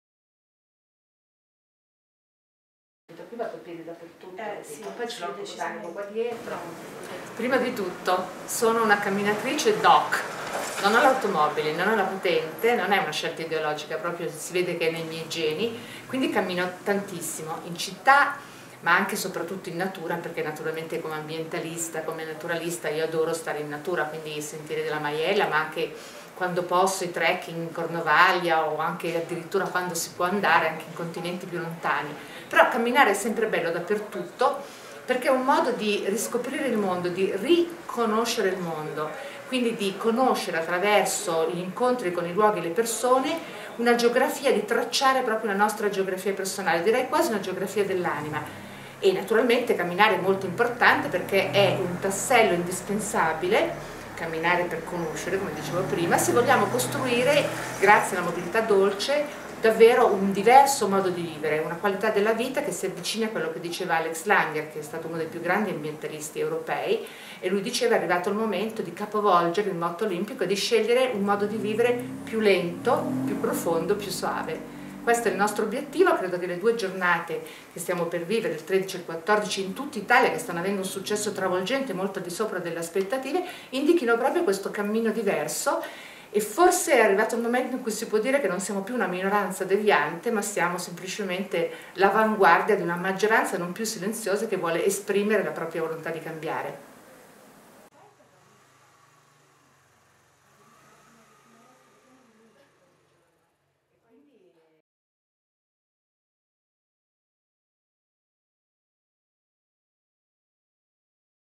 Grazia Francescato - Giornata Nazionale del Camminare
Intervista a Grazia Francescato, ambientalista ed ex Presidente del WWF e dei Verdi, Testimonial dell'Evento.